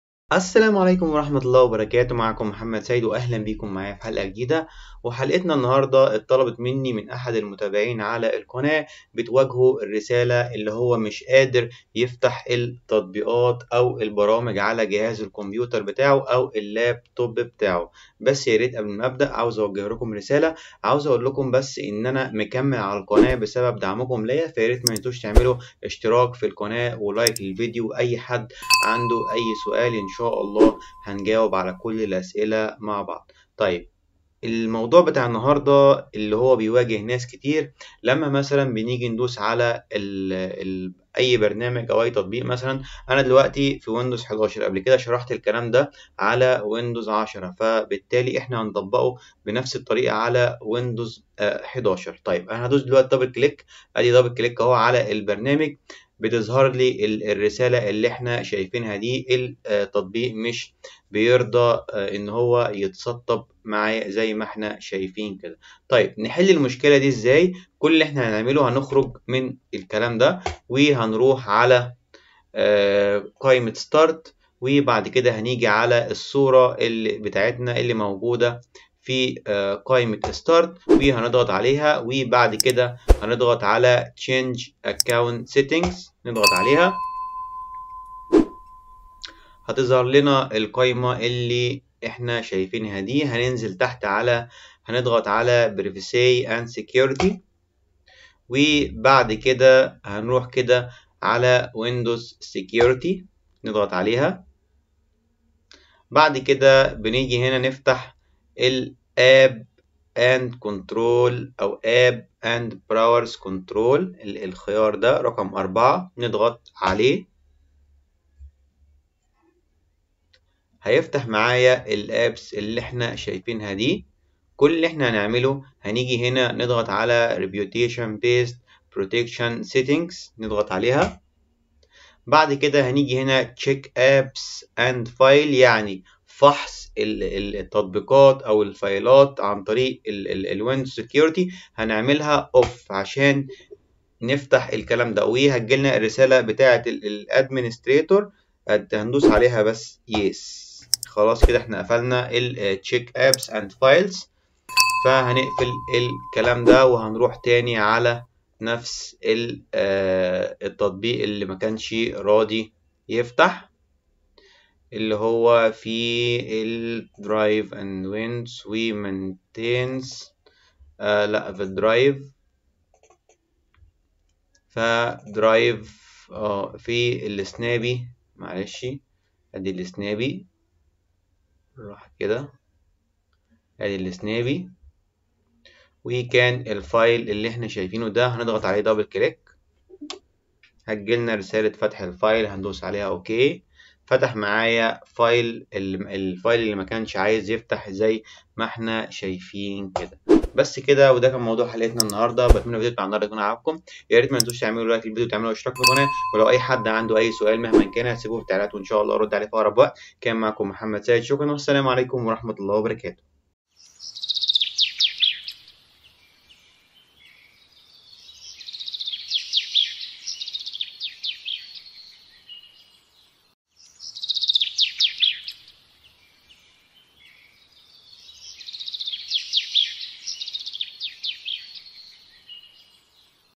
حل مشكلة windows protected your pc | حل مشكلة windows protected your pc no run anyway في ويندوز 11
في هذا الفيديو حل مشكلة رسالة windows protected your pc اثناء فتح التطبيقات على الكمبيوتر عن طريق خطوات بسيطة من خلالها نقدر نلغي الرسالة ونقوم بفتح التطبيقات والبرامج على الكمبيوتر بسهولة وتعرف المشكلة بأن تطبيق Windows Security يقوم بفحص التطبيقات ويرفض فتحها لذلك شرحنا في هذا الفيديو طريقة اغلاق فحص التطبيقات لكي نقوم بفتح البرامج والتطبيقات على جهاز الكمبيوتر بسهولة